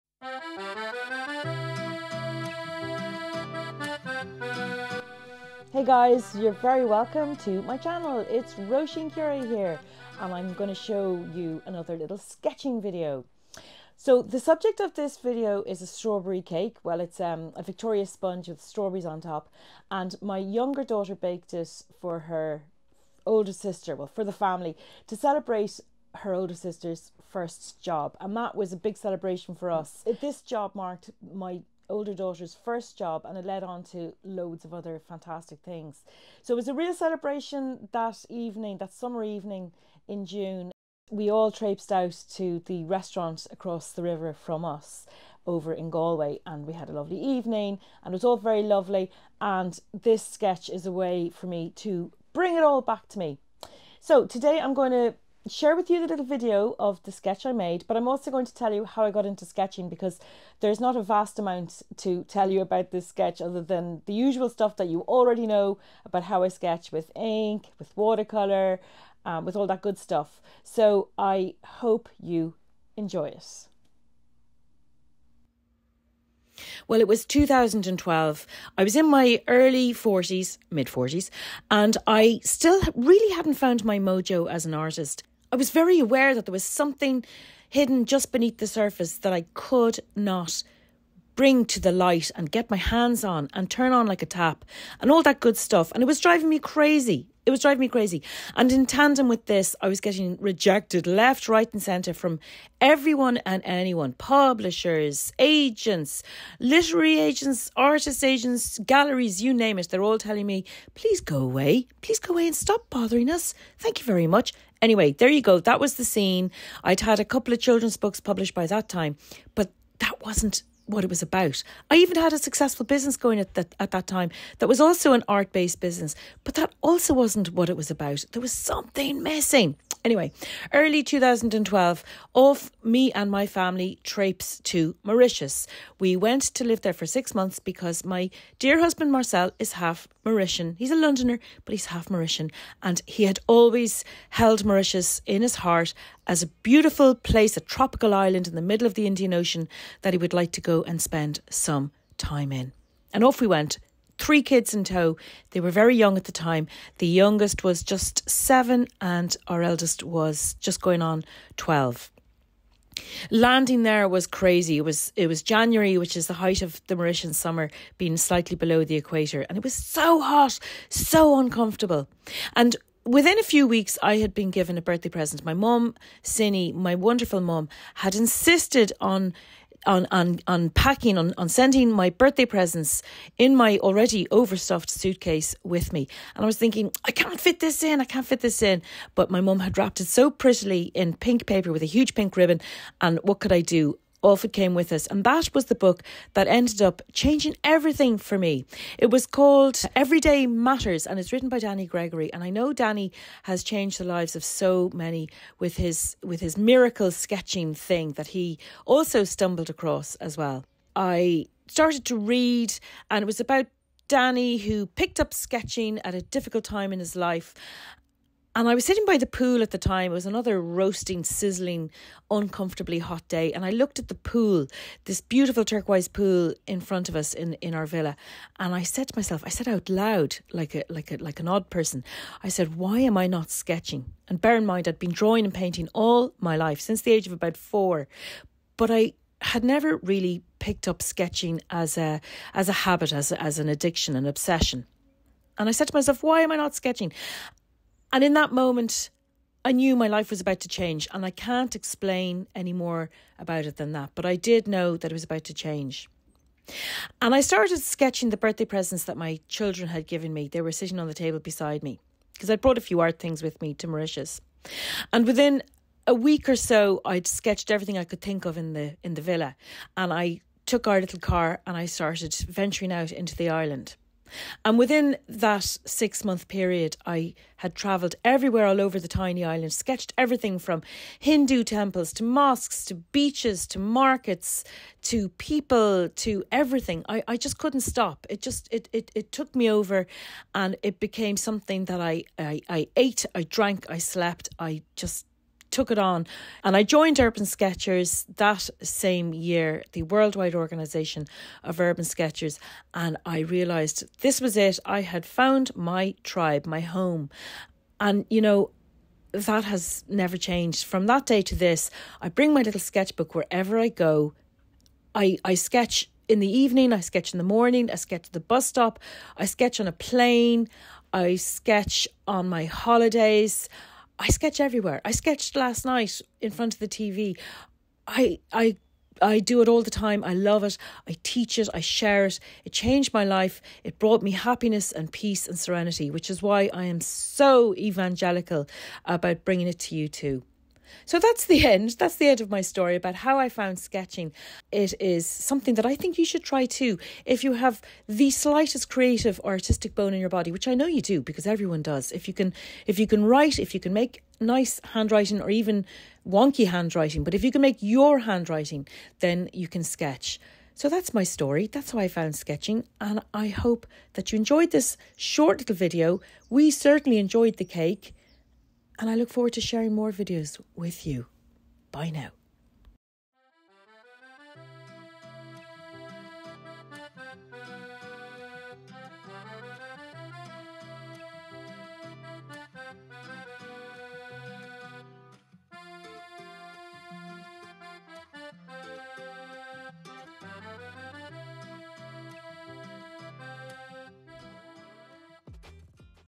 How I Discovered Sketching - And A Sketch Of A Cake With Strawberries On Top
It's twelve years now since I discovered sketching. By that I mean everyday sketching, everywhere sketching, making art of the world around you part of your daily life. Even though I had been an artist - and a woking one, at that - since the age of fourteen, I had never carried around my sketching stuff with me as a matter of course.

All that changed in 2012 when my family and I made the bold move to live on a tiny island in the middle of the Indian Ocean for six months. Mauritius is many things: a honeymoon destination, a tropical paradise, a peaceful country, a centre of business entrepreneurship from the single old lady selling a couple of mangoes on the roadside to tech start-ups. And it's where my passion for sketching was born simply because my mother insisted that I would open her birthday gift to me while I was there. The gift, a prettily-wrapped, pink be-ribboned book I stuffed into my suitcase, ended up changing my life. (Mothers! They are our personal angels.)

I started by sketching the birthday gifts my kids had given me and by the time we left the island I had sketched everything from religious buildings - there are a lot of them in Mauritius - to markets and turquoise lagoons, to the Mauritians who came and stared at my sketching activities and entertained me with their chatter. I even wrote a memoir about my time there, which I still hope to publish. (That was really my first book, even though it remains a manuscript on a hard drive somewhere.)

Sketching changed everything for me. It is the reason I have a career, but much more than that it's brought me serenity and a deep satisfaction. We all need to express ourselves as humans, and for some reason sketching fits the bill for me. I know it does for so many of you too, and that's the WHY of my evangelical passion for teaching.

There'll be lots more stuff about my time in Mauritius because you';ve told me you would like to hear more - if that's you too, then you should subscribe to my blog, which is on my website, roisincure.com.

I hope you enjoy the video of sketching the yummy cake too!

Feeling quite peckish now :)

Róisín